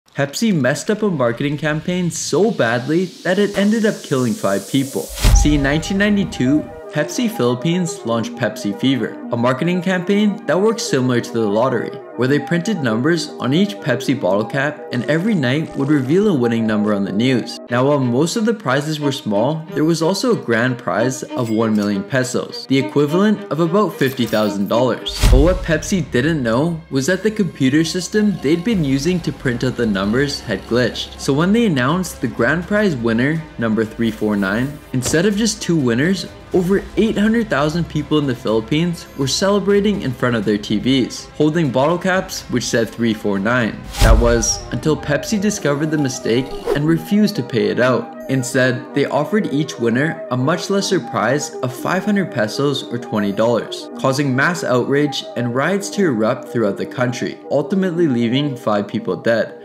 The Pepsi Contest That Killed 5 People...
Pepsi launched a marketing campaign that they probably never want you to hear about.